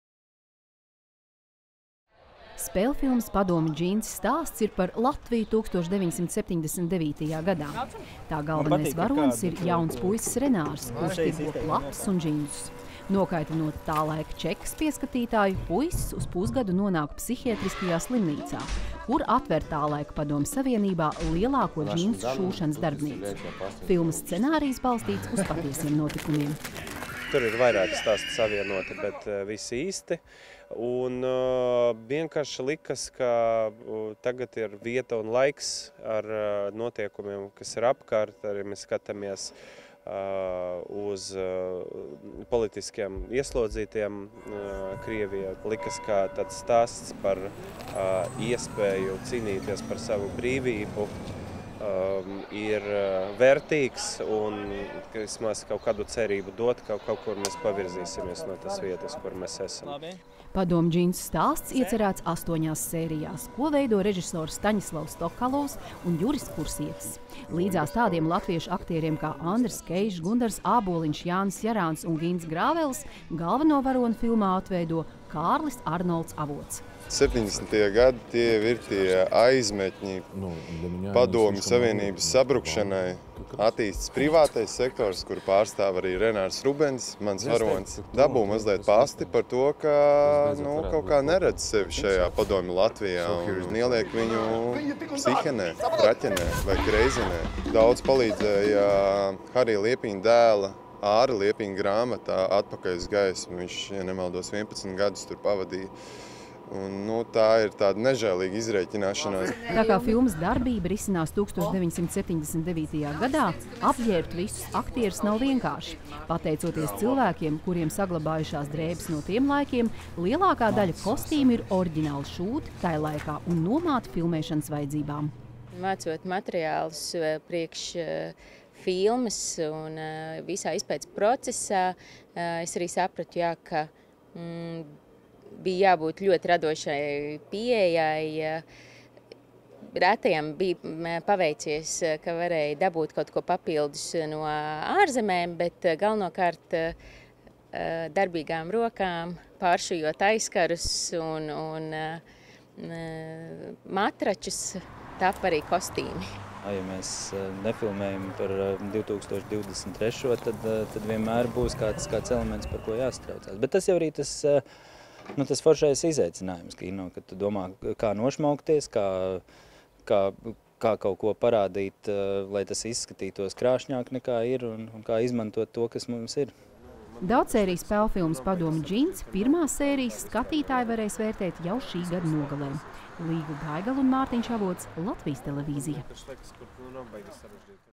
Turpina darbu pie daudzsēriju spēlfilmas “Padomju džinsi”
Latvijā tiek uzņemta jauna daudzsēriju spēlfilma “Padomju džinsi”, kas vēstī par pagājušā gadsimta 70. gadu beigu notikumiem Latvijā . Filmai iecerētas astoņas sērijas un to veido režisori Staņislavs Tokalovs un Juris Kursietis. Tas būs stāsts par brīvības cenu un vērtību, kas ļoti aktuāls tieši aktuālo pasaules notikumu kontekstā. Filmēšanas laukumu apmeklēja Līga Gaigala.